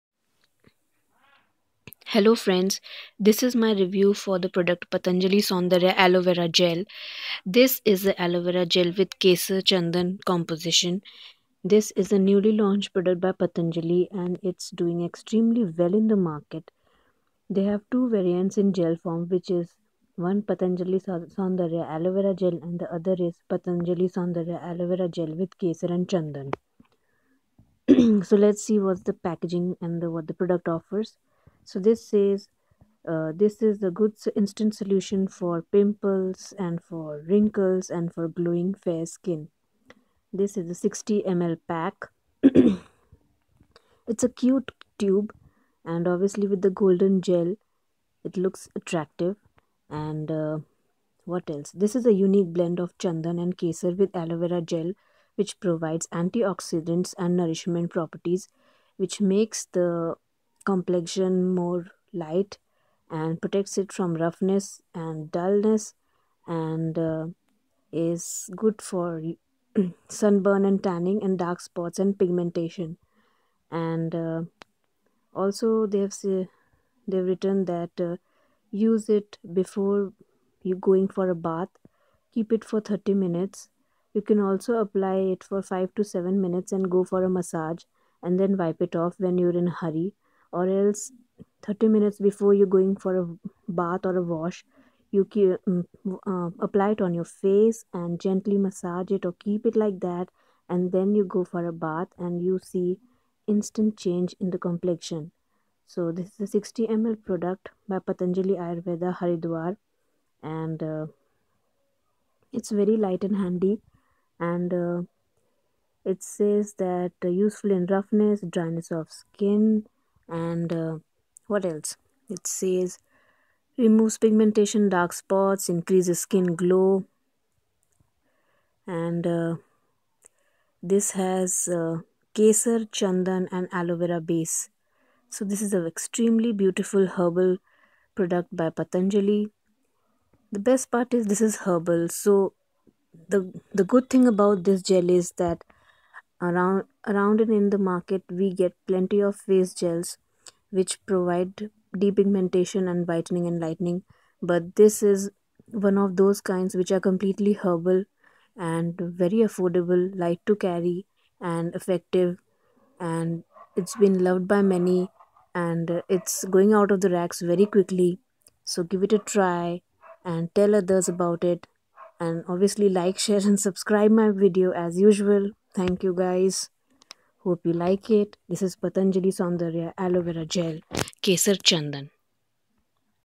Patanjali Saundarya aloe Vera gel with kesar,chandan.
Patanjali Saundarya aloe Vera gel with less and chandan for glowy,fair,soft n smooth blemish free skin,60 ml,50 Rs.